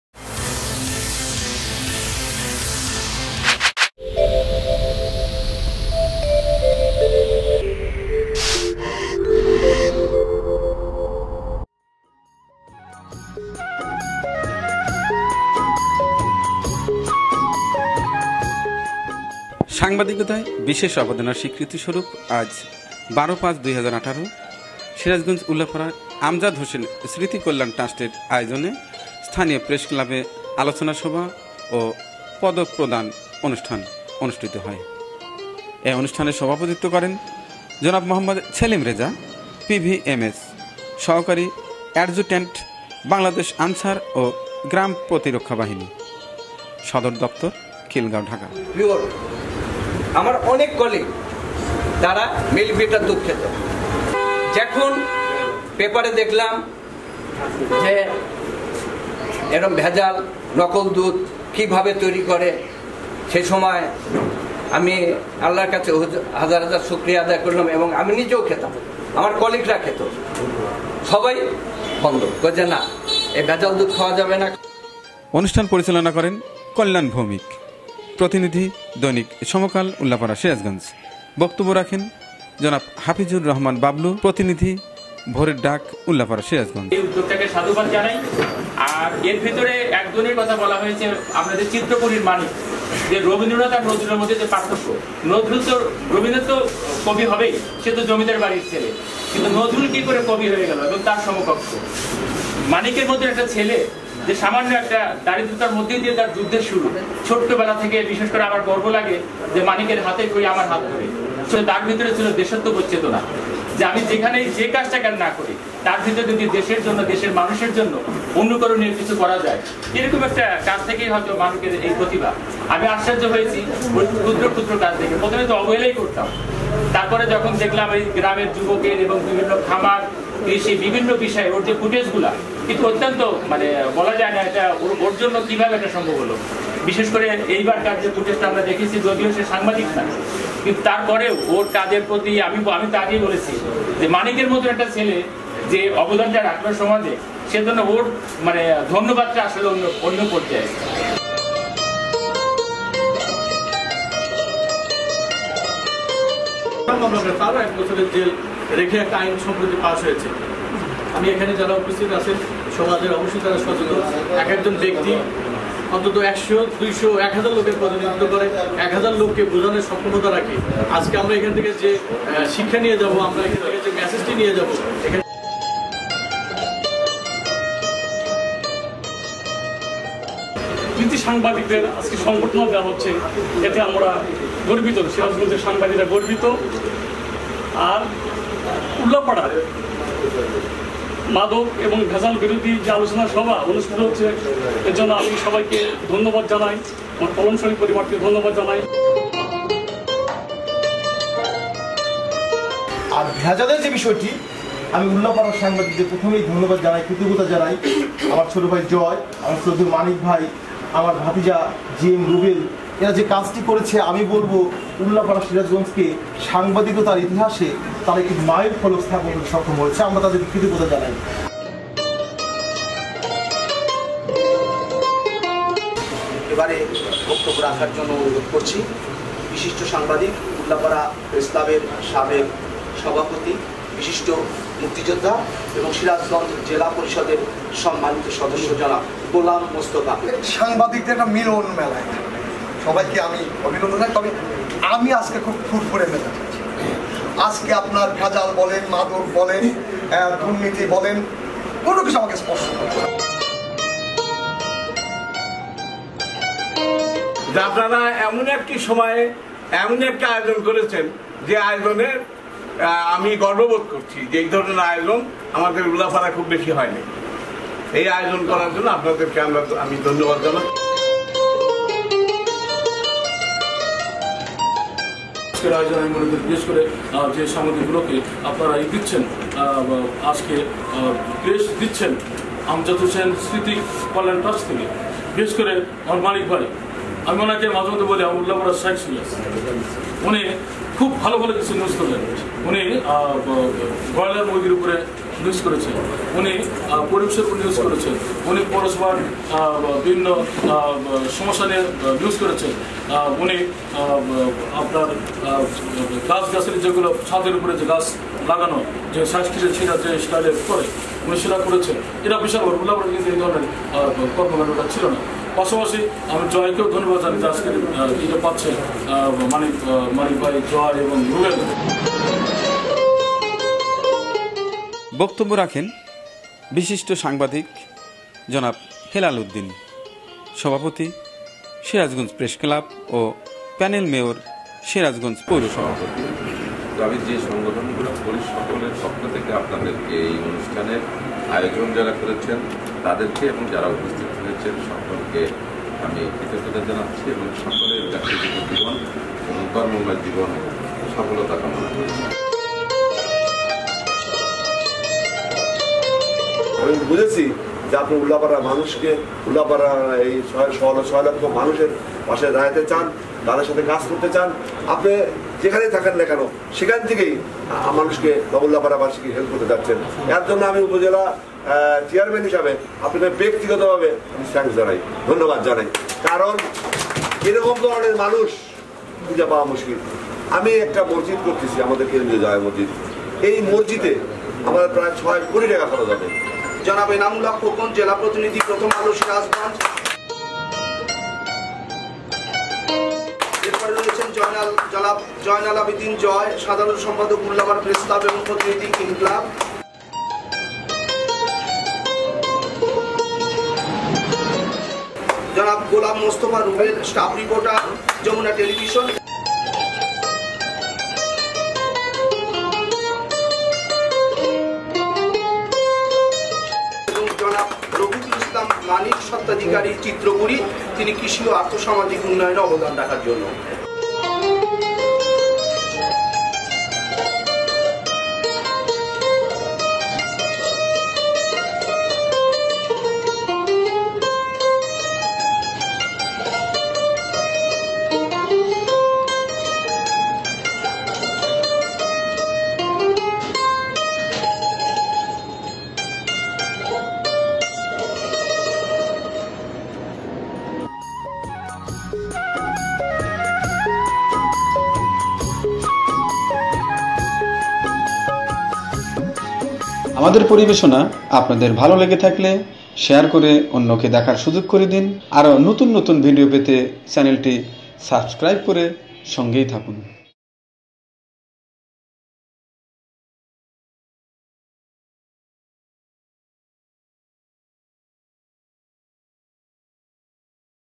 কৃষি ও আর্থ-সামাজিক উন্নয়নে অবদান রাখায় পুরষ্কৃত হলাম।
পদক,  সকলের ভালোবাসায় কৃষি ও আর্থ-সামাজিক কাজে অবদানের জন্য “আমজাদ হোসেন স্মৃতি কল্যাণ ট্রাষ্ট” পদক পেলাম ।সবাই দোয়া করবেন যেন আগামীতে আরো ভালো কাজ করতে পারি।সবাই কৃষিচিত্র দেখুন পরামর্শ দিন ।